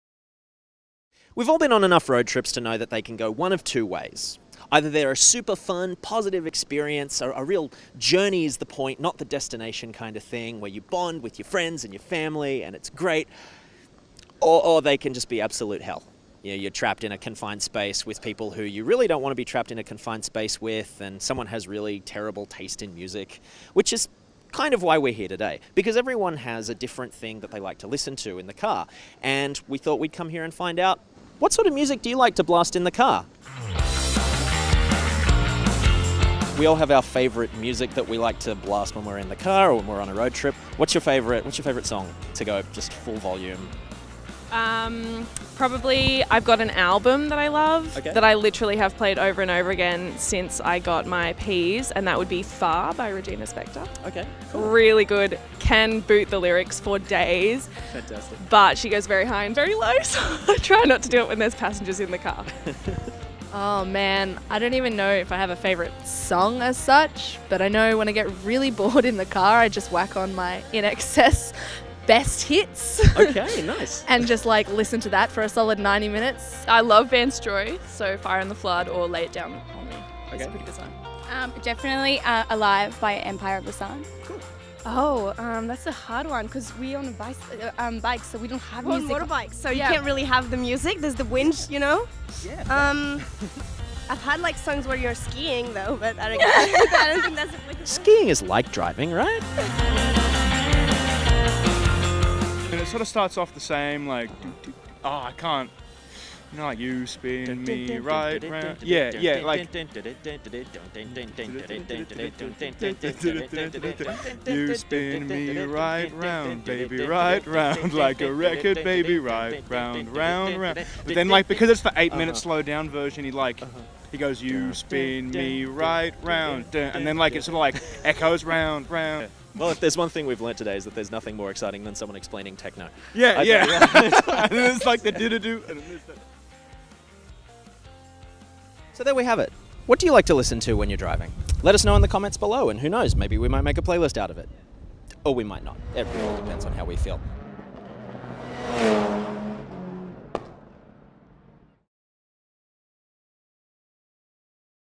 Vox Pop: The best music to drive to
We were curious as to what people's favourite songs to drive to were, so we sent out a guy with social anxiety to talk to you - the people - and find out: what's the best song to blast when you're driving?

Oversteer - For car nuts and speed readers